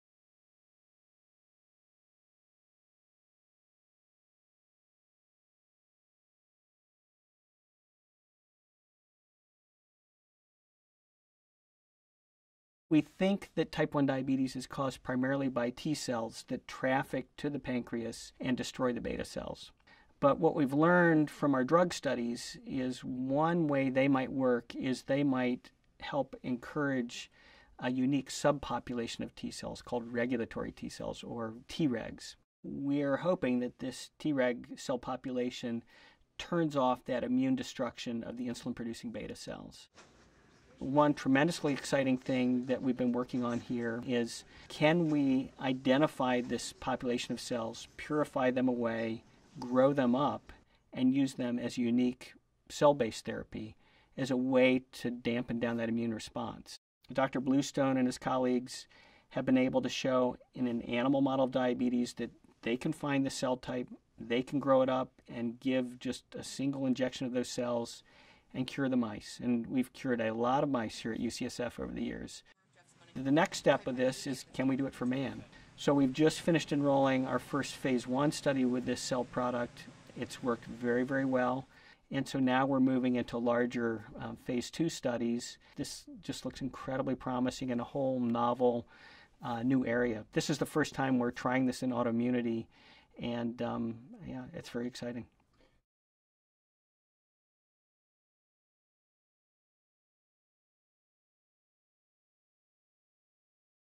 A groundbreaking clinical trial
Stephen Gitelman, MD, describes how research in regulatory T-cells led by Jeff Bluestone, PhD, has opened a doorway to treating type-1 diabetes with cell-based therapy.